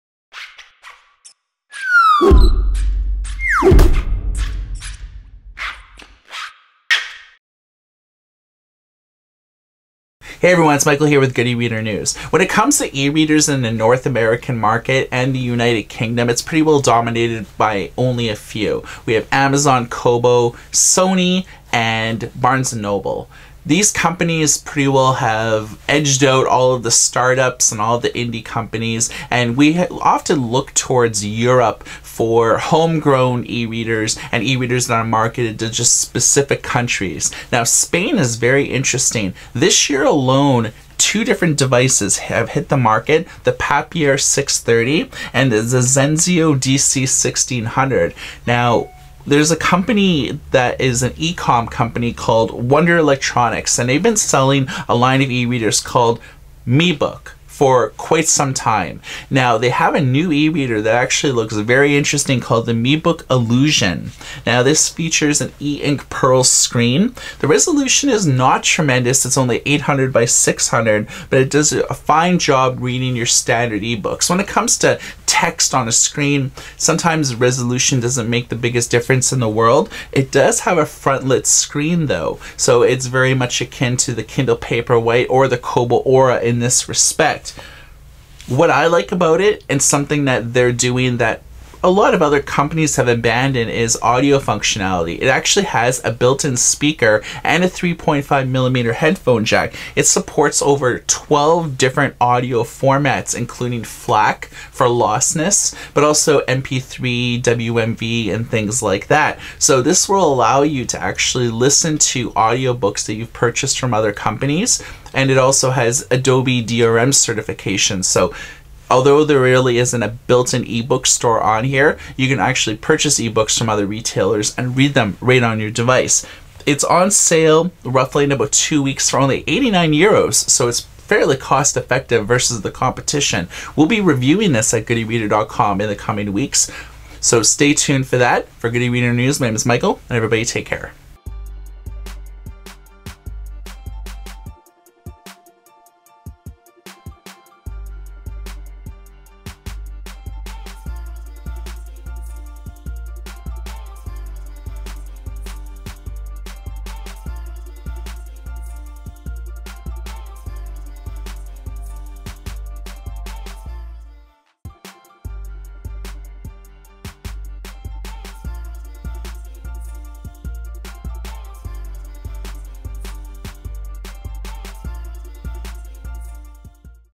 miBuk ILUSION e-Reader Announced
The miBuk ILUSION features a six inch e-Ink Pearl display screen with a resolution of 800×600. It will do a fine job in reading your standard eBooks, but might struggle with more high-definition content, such as PDF magazines and newspapers. This reader was designed to read in the dark and has a front-lit display. Underneath the hood is a Allwinner Processor E200 processor, 4 GB of storage and an SD card to 32GB of additional memory.